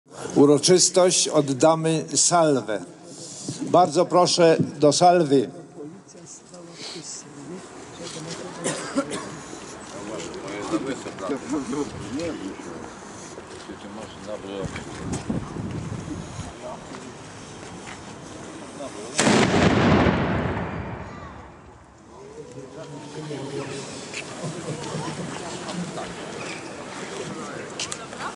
Jaku Wejher -359 rocznica śmierci
Jaku Wejher -359 rocznica śmierci ,załozyciela miasta Wejherowa no i Wejherowskiej  Kalwarii